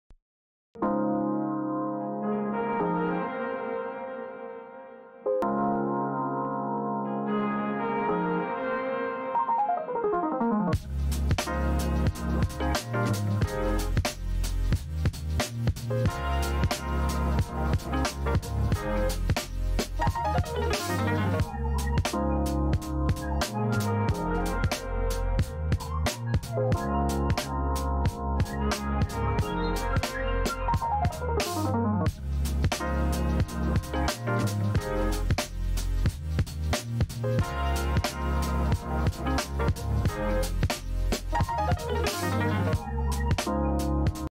Corvette C1
Corvette C1 currently for sale at Autostorico.